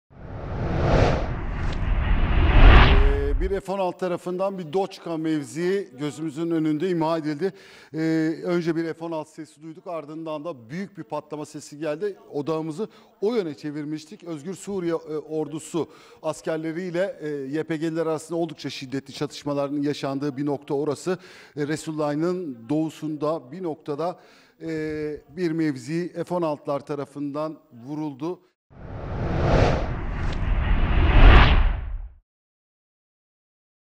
Bir doçka mevzii F-16 ile yok edildi (NTV ekipleri sınır hattında)
Türkiye'nin güney sınırında oluşturulmaya çalışılan terör koridorunu yok etmek, bölgeye barış ve huzuru getirmek amacıyla yürütülen Barış Pınarı Harekatı kapsamında, Tel Abyad ve Resulayn'daki terör hedefleri sabahın erken saatlerinden itibaren obüs atışlarıyla vuruluyor. F-16 savaş uçağı tarafından bir doçka mevkii vurularak yok edildi. NTV ekibinden Gökhan Gerçek ve Yasin Gündoğdu canlı yayında Resulayn'dan son durumu aktardı.